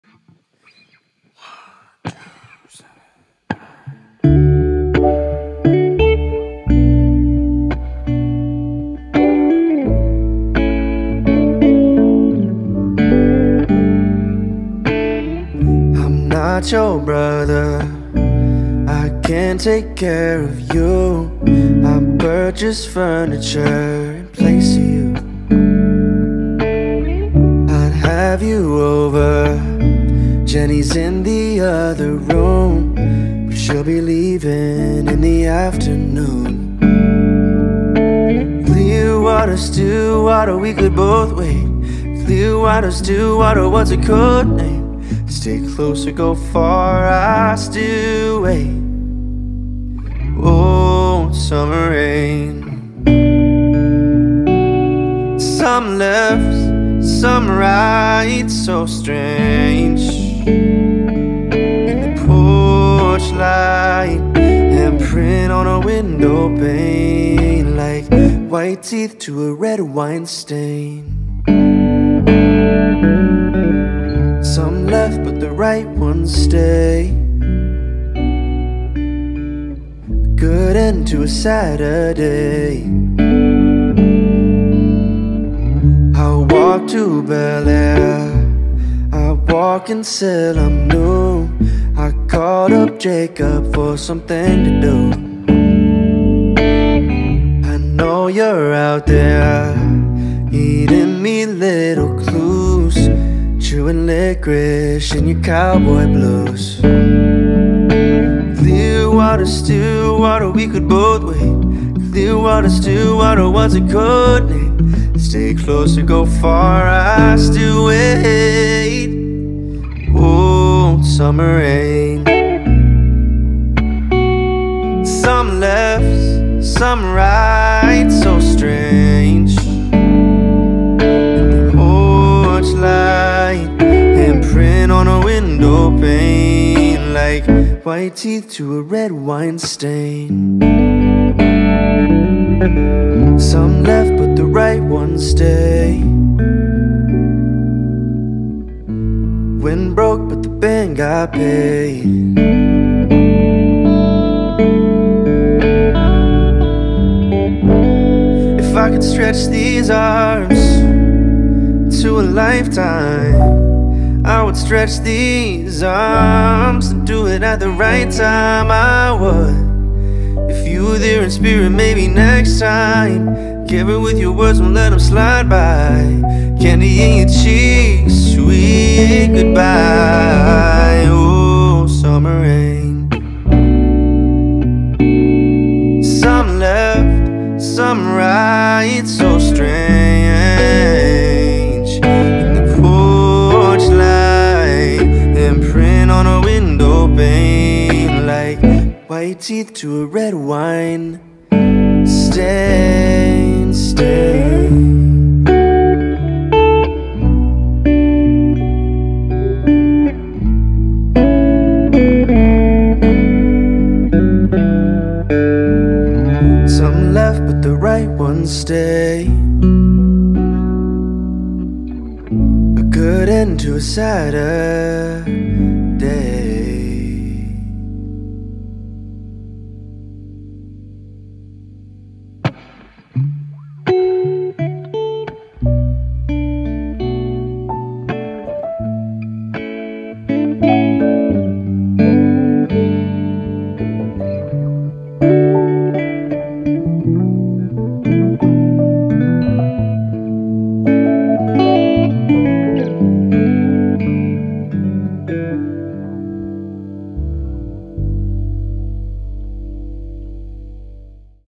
covering my favorite Ryan Beatty song
felt like switching it up today on main so here's my cover of my favorite ryan beatty song